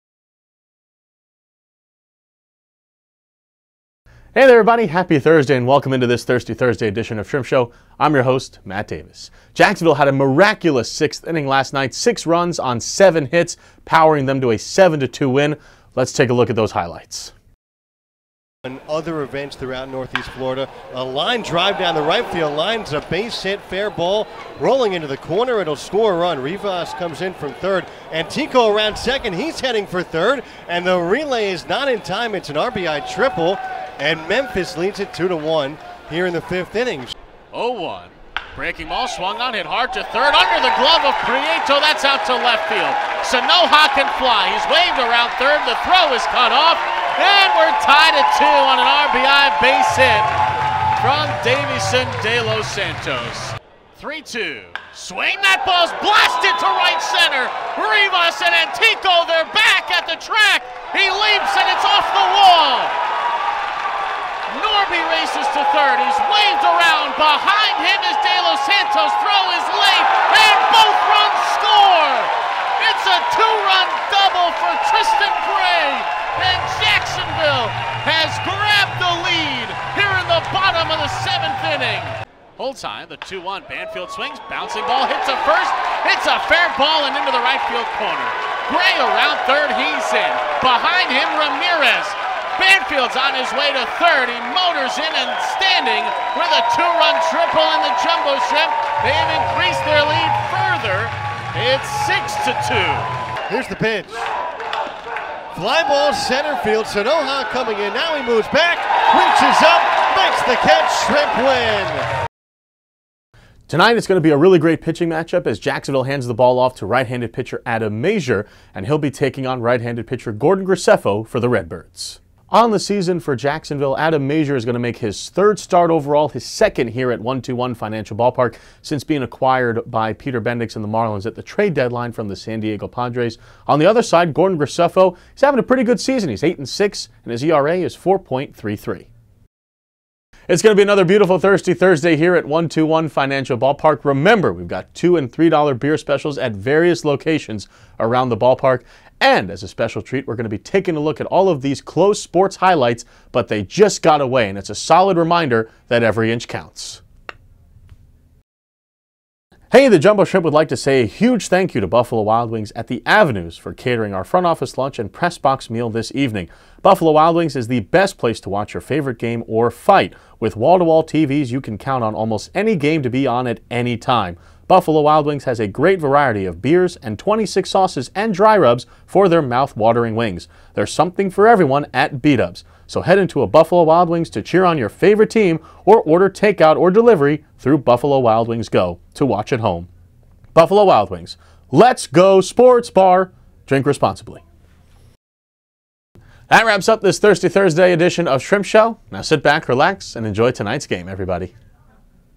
Shrimp Show August 15
It's another Coors Lite Thirsty Thursday here at 121 Financial Ballpark as we play game three of the Memphis Redbird's series! Join our broadcaster, Matt Davis, as we take a look back at yesterday's win and get ready for beer and baseball!